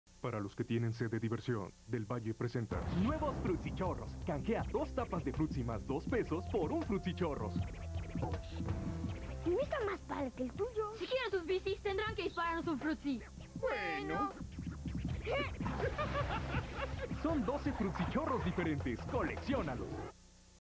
Comerciales mexicanos: Frutsi Chorros 2002
Si se queria un frutsi chorros se tenia que dejar a la tiena 2 tapas de frutsi con 2 pesos para el funcionamiento se tenia que apretar el frutsi y de ahí sale el chorro